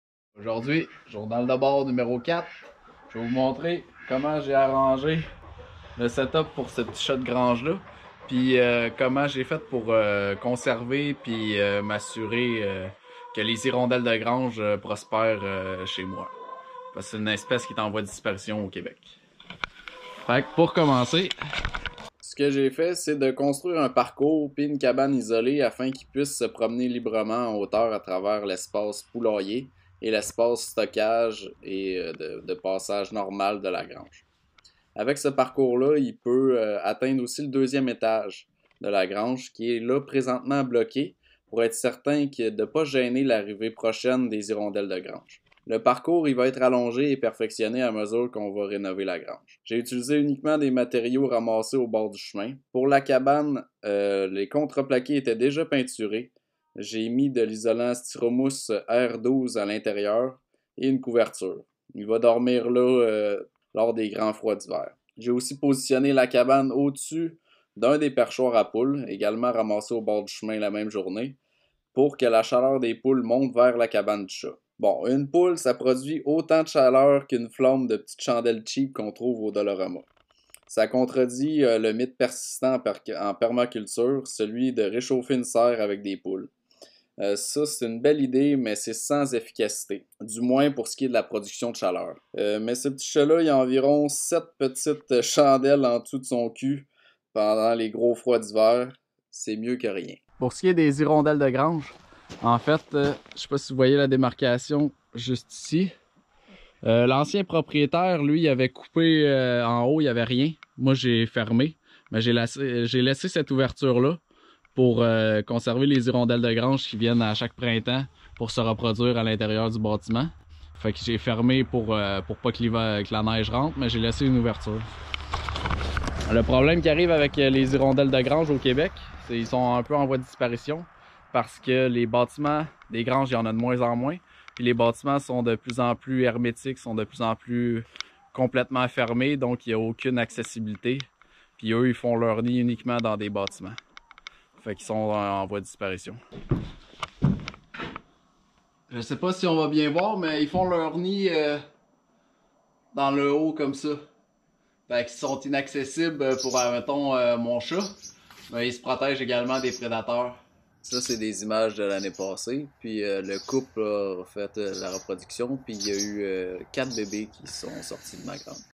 Parcours à Chat + Hirondelle de Grange !!!
Bref JDB: les installations pour notre chat ont été fait l'année passée de même que la conservation de l'ouverture afin que les hirondelles de grange continuent de nicher chez nous !

0:00 Intro
0:22 Parcours pour notre Chat Bolivar !
1:03 Explications et déboulonnage du mythe en permaculture (des poules pour chauffer une serre)
1:40 Conservation des Hirondelles de grange !

Le meilleur moyen pour aider financièrement la chaîne, montrer votre soutien et avoir accès en primeur aux videos

Expérimentation sur 1an
Filmer et montage : 4hrs